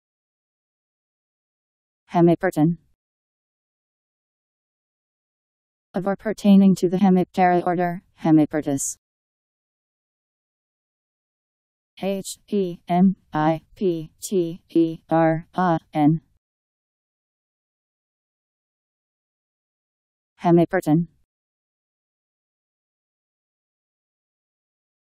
Hemipteran Meaning
Video shows what hemipteran means. Of or pertaining to the Hemiptera order, hemipterous.  Hemipteran Meaning. How to pronounce, definition audio dictionary. How to say hemipteran. Powered by MaryTTS, Wiktionary